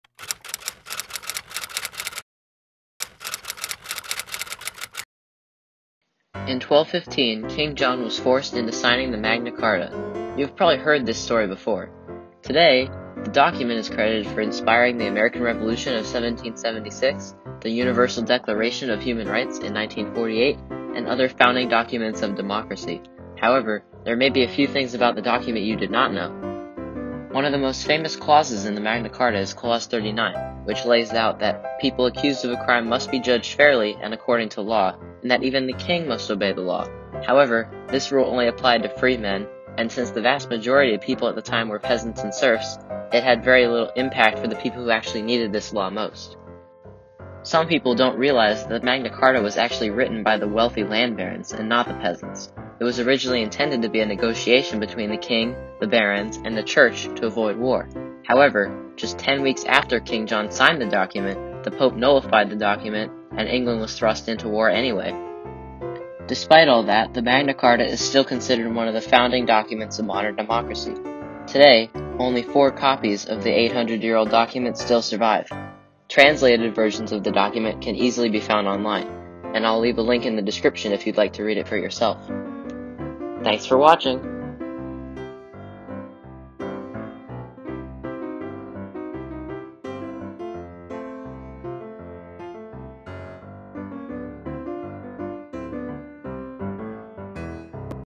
Magna Carta: An Overview
The Magna Carta is a very important document for the history of Democracy, however, there may be some things you did not know about it.

Thumbnail picture:
Medievalists.net